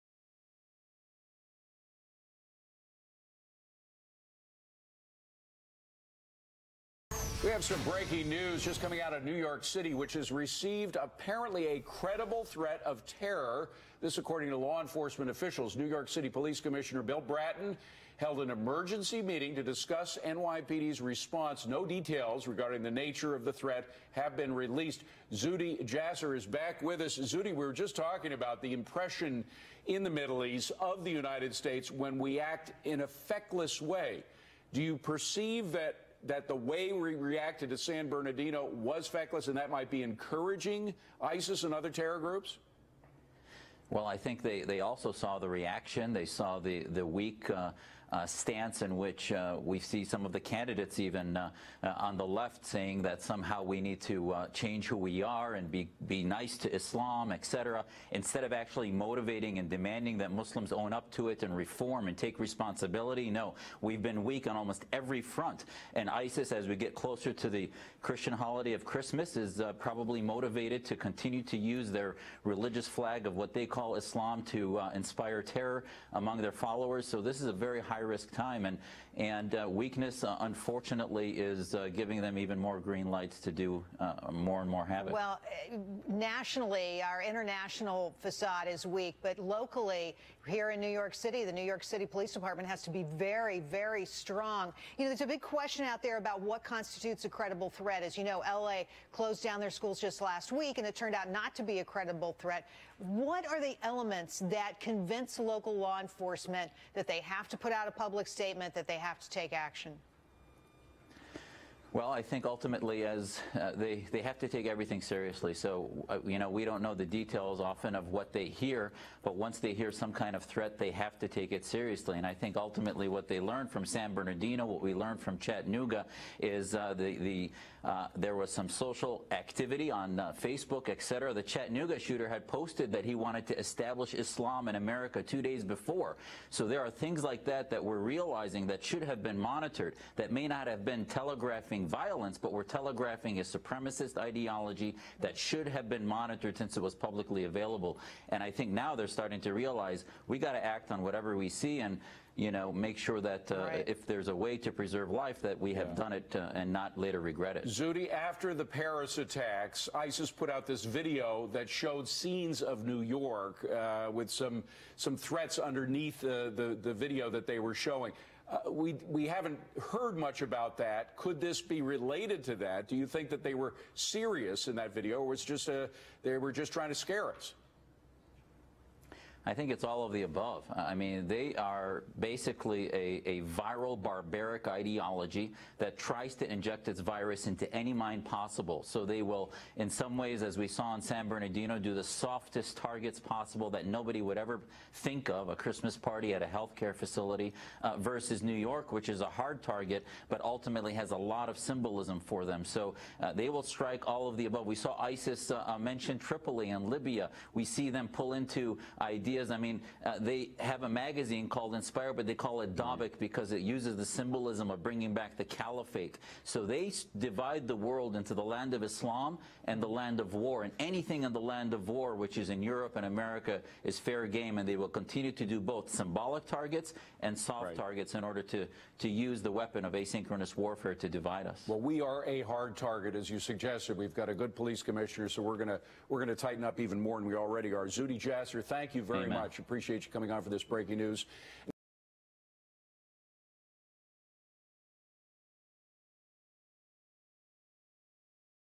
Dr. Jasser reacts to breaking news of a credible threat against NYC 12.22.2015
Dr. Jasser reacts to breaking news of a credible threat against NYC and how the current temperament against ISIS and radical ideology within Islam plays to this threat.

Join the conversation on Twitter @AIFDemocracy @DrZuhdiJasser